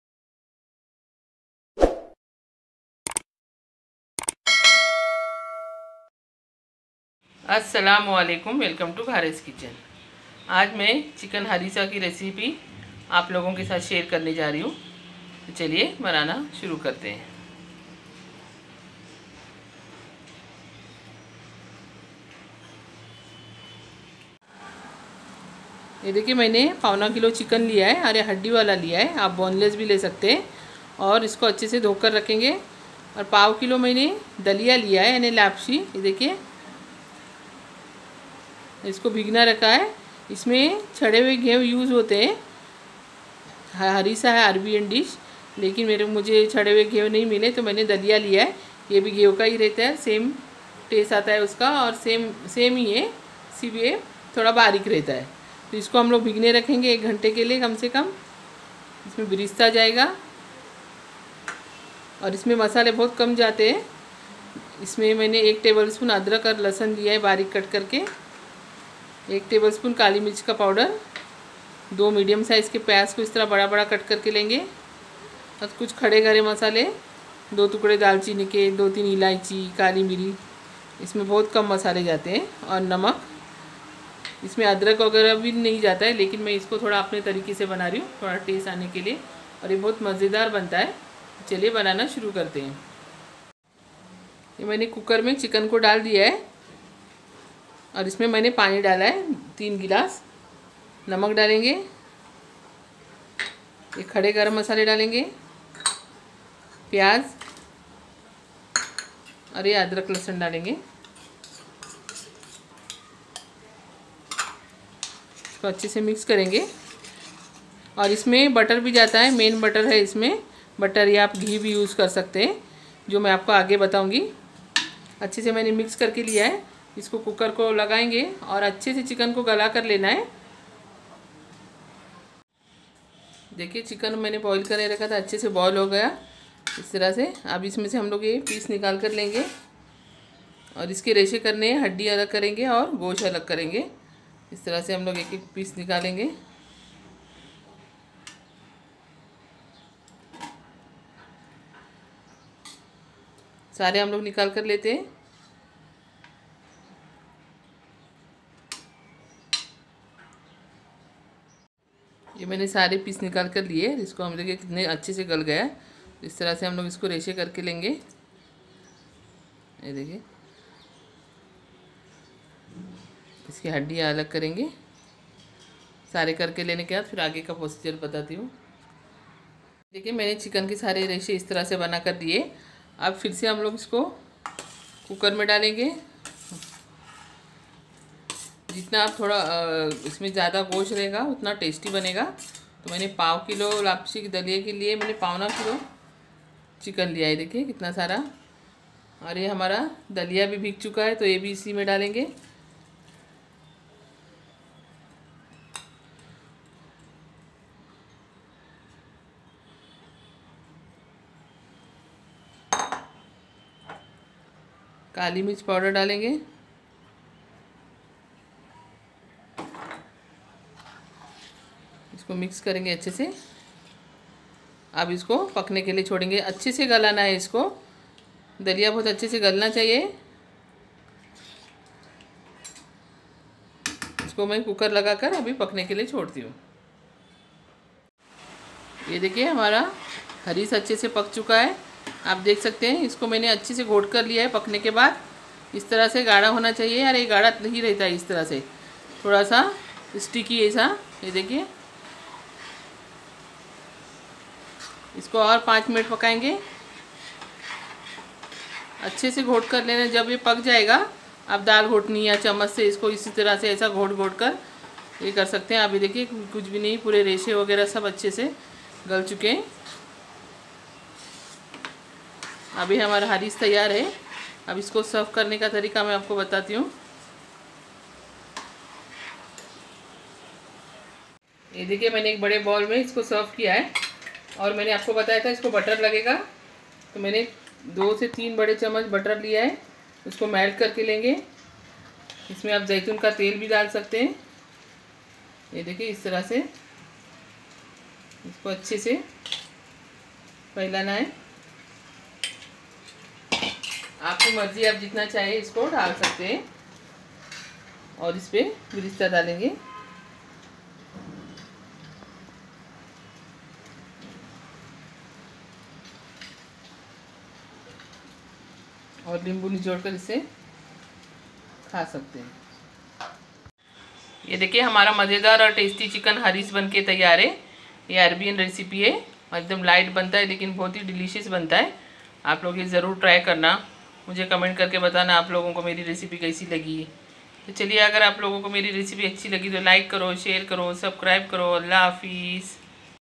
Chicken Harissa Recipe | Arabian Chicken Hareesa | Ghare's Kitchen
Assalamualaikum

Aaj mai chicken harissa ki recipe aapke sath share kar rahi hu

Ingredients:

750 grams chicken
250 grams laapshi
Birista
1 tbsp ginger & garlic chopped
1 tbsp black pepper powder
salt as per taste
mix akkha garam masala
2 Onions
Butter / OIl